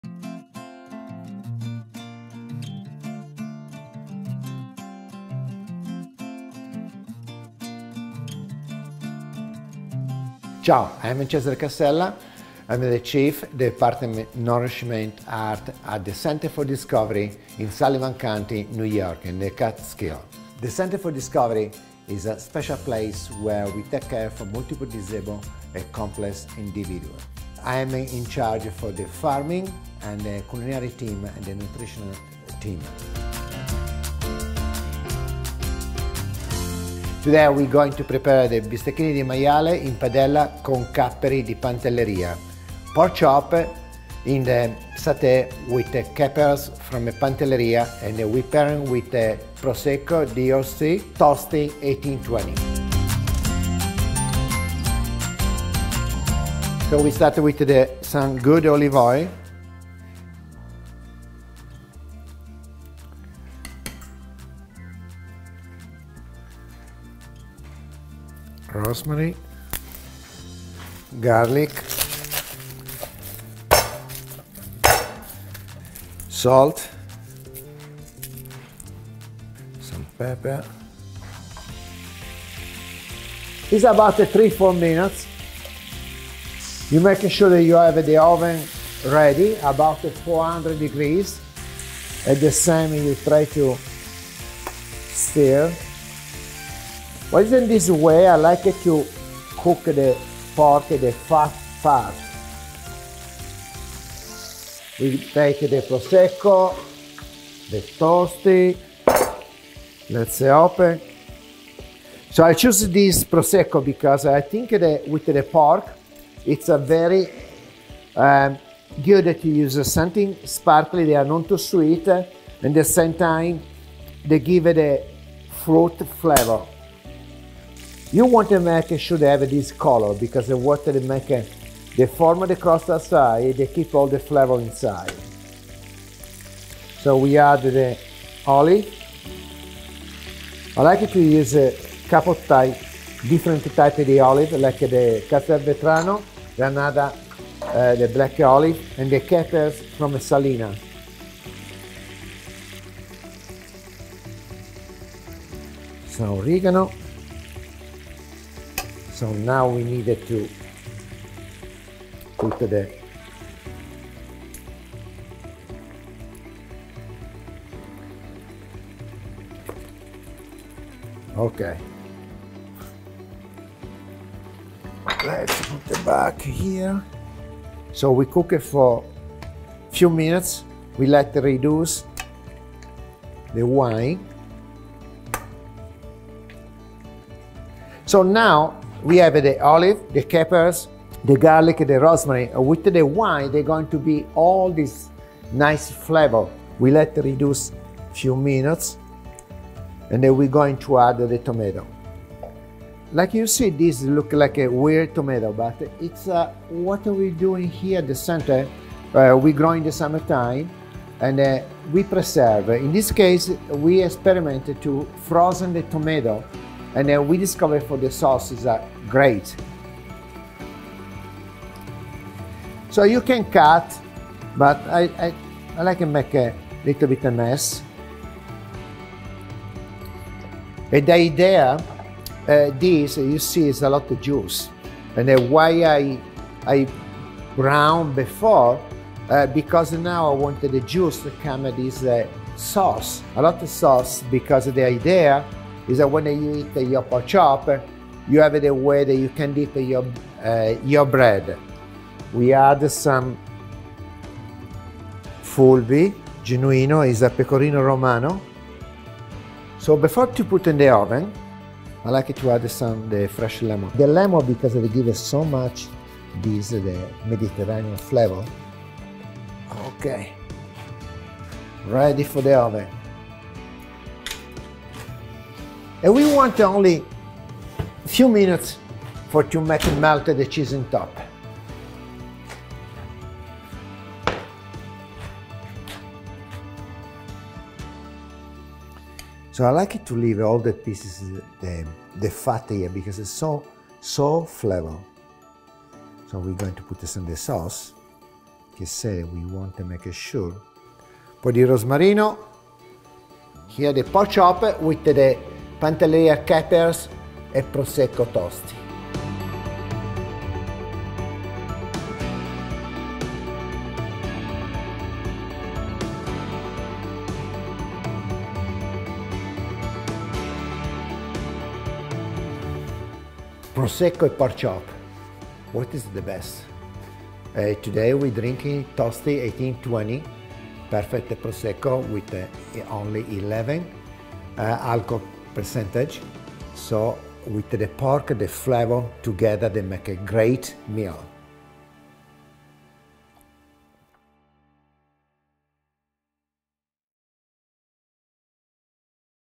Bistecchine di maiale con capperi di Pantelleria – Cesare Casella, New York
🇮🇹 Responsabile del Department of Nourishment Arts presso il Center for Discovery, a Sullivan County, Cesare Casella supervisiona un team di chef, nutrizionisti, terapisti, agricoltori ed educatori. Da oltre 35 anni si impegna a far conoscere la vera cucina italiana a New York. Oggi prepara per noi delle bistecchine di maiale in padella con capperi di Pantelleria, abbinandole ad un Prosecco DOC Extra Dry Tosti 1820. Il perlage fine e persistente e le note aromatiche conferiscono al piatto il giusto equilibrio, donandogli un gradevole gusto fruttato.

🇬🇧 Cesare Casella is the Chief of the Department of Nourishment Arts at the Center for Discovery in Sullivan County. He supervises a team of chefs, nutritionists, therapists, farmers and educators. He has been committed to raising awareness about authentic Italian cooking in New York for more than 35 years.  Today he is preparing sautéed pork chops with capers from Pantelleria which he has paired with a Prosecco DOC Extra Dry Tosti 1820. The fine and consistent perlage and fragrant notes give balance to the dish through a pleasant fruity flavor.